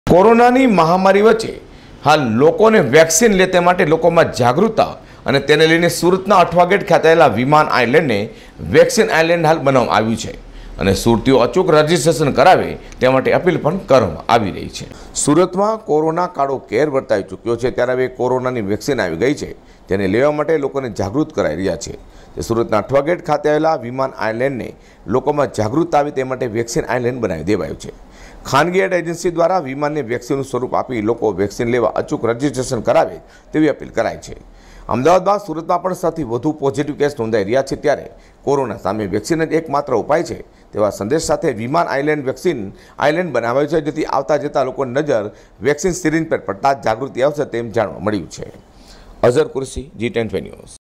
સુરતમાં વેક્સિનેશન લેવા જાગૃત્તા માટે અનોખી પહેલ | G 24 NEWS
અઠવાગેટ વિમાનને વેક્સિન આઈલેન્ડ બનાવાયું